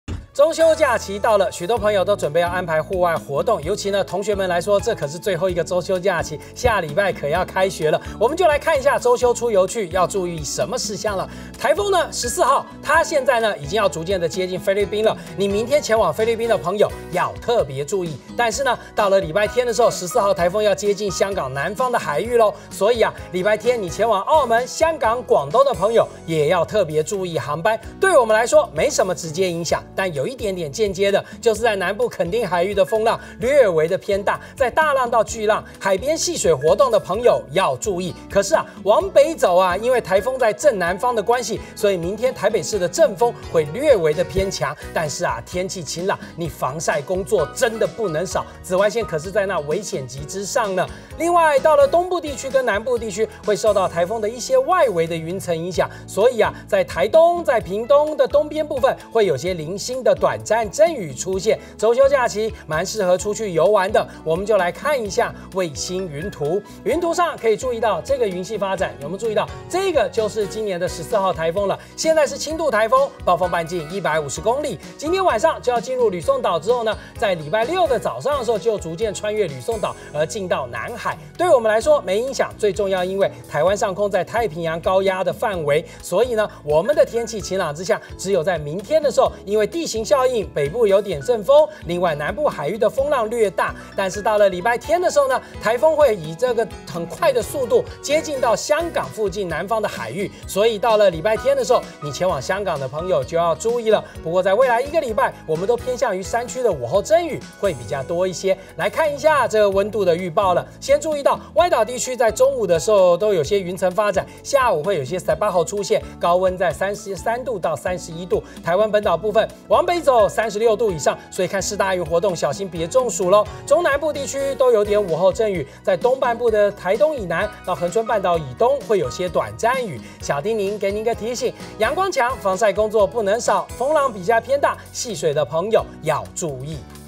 20170825中天新聞 【氣象】14號颱風過菲國 前往旅遊注意航班
颱風週日接近香港，港澳航班要注意，南部海域風浪大，海邊戲水注意安全，周休往北天晴炎熱，往南局部短暫雨，中部以北陽光強，戶外活動要防曬。